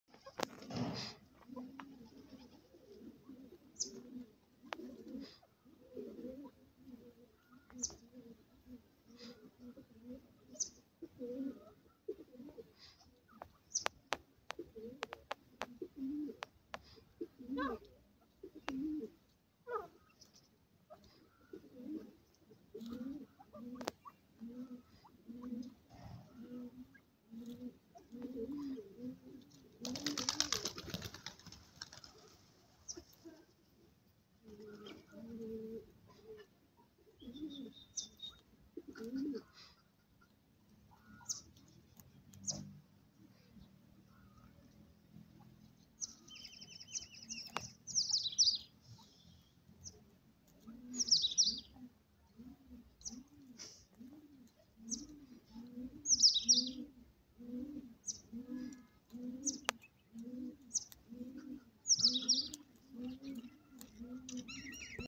gora teetar,pheasant ringneck birds in the forest
Gora teetar, also known as pheasant ringneck birds, are a fascinating group of birds that live in the heart of the forest
 Come along with us on an exciting journey into their world
In this enamoring video, we dive profound into the rich vegetation of the woods, where these heavenly birds meander openly in the midst of the serenity of nature
 Witness their staggering plumage and elegant developments as they search for food and collaborate with their environmental factors

As energetic untamed life lovers, we're committed to imparting the excellence of nature to the world, and this experience with the gora teetar is a genuine demonstration of the marvels of the normal world